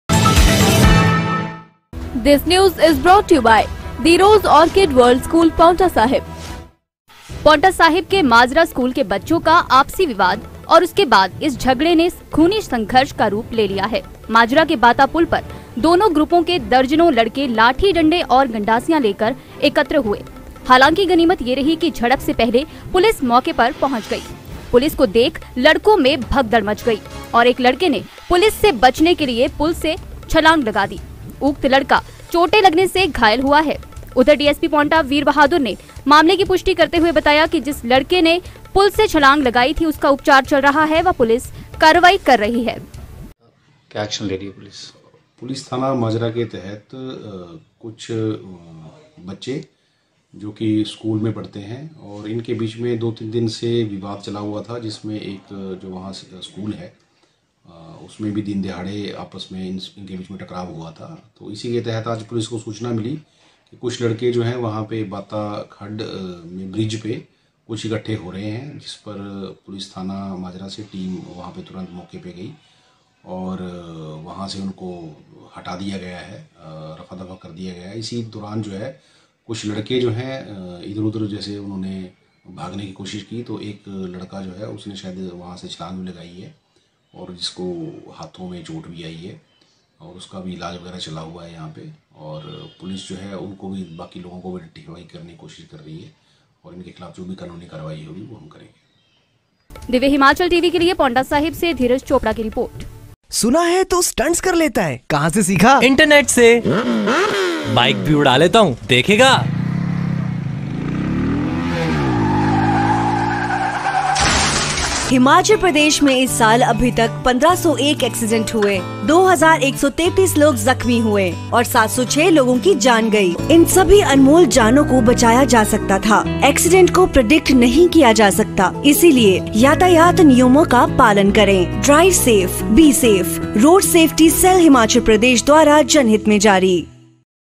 खूनी झड़प और पुलिस से बचने को युवक ने पुल से लगा दी छलांग
HimachalNews: खूनी झड़प और पुलिस से बचने को युवक ने पुल से लगा दी छलांग

divyahimachaltv
dhtv,tvnews
local news
divyahimachalgoodmorning
khabre ftafat
divyahimachalgoodafternoon